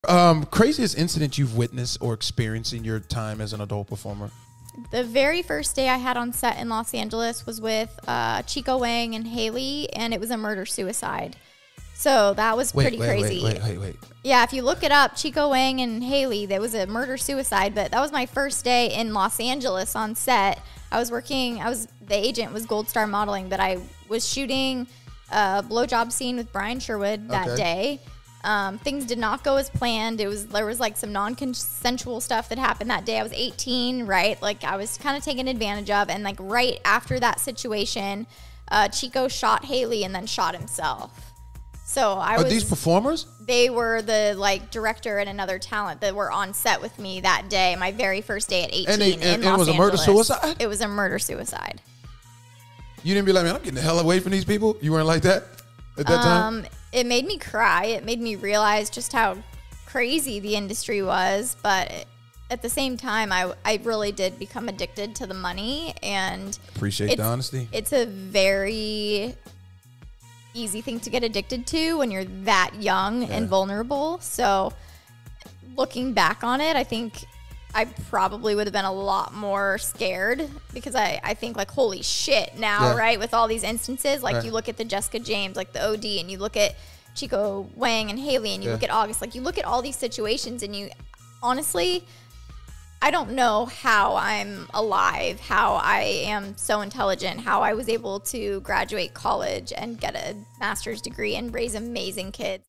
Madison Scott I Was On Set The Day Chico Wang Killed His Wife Haley Paige And Then Committed Suicide
Check Out A Clip From Madison Scott Interview On @TheBougieShow Madison Shares With Xay That Her First Time Filming At The Age Of 18 In Los Angeles California, Was The Same Location And Time That Chico Wang Killed His Wife Haley Paige And Then Committed Suicide On Himself.  Interview Premiering October 14th At 6pm On @TheBougieShow Channel.